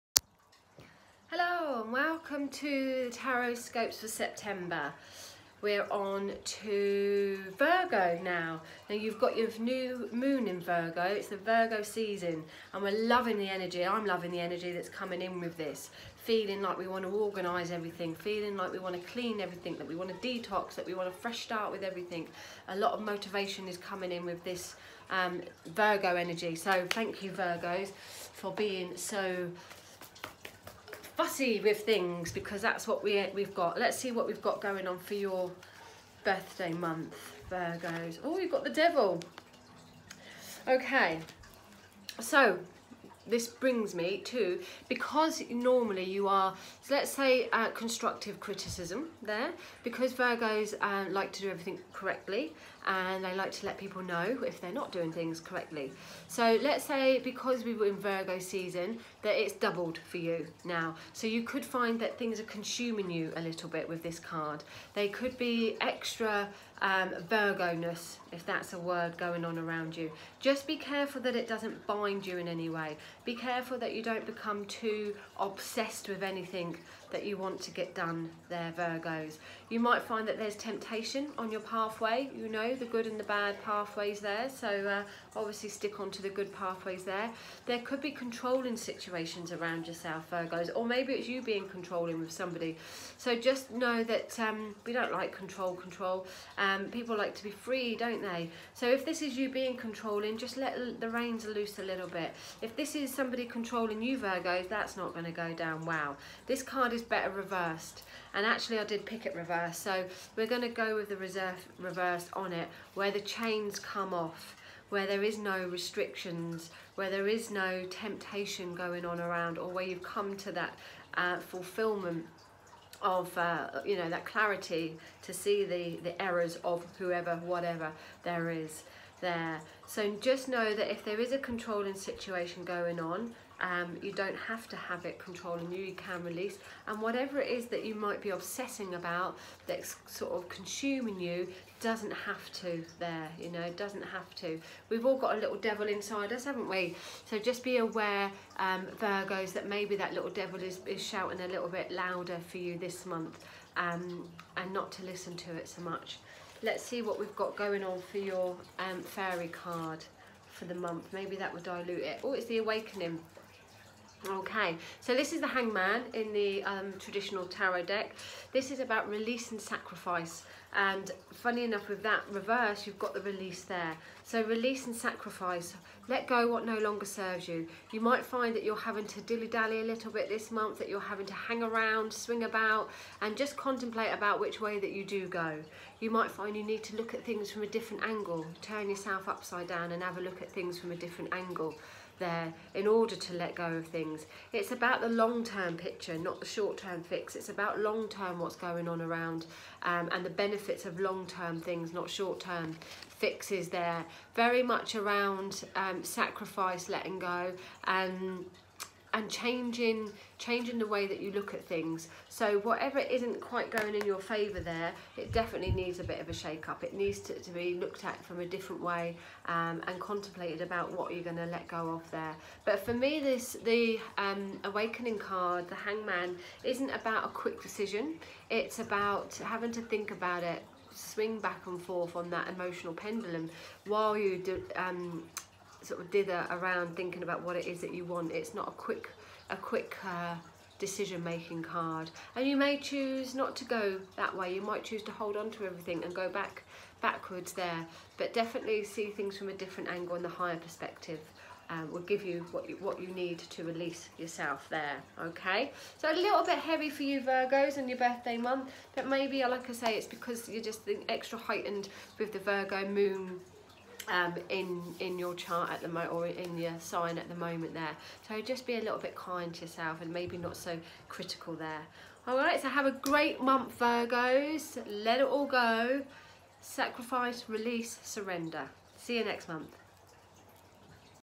Tarot scopes for September for Virgo
See what’s happening for Virgos in Septembers tarot scopes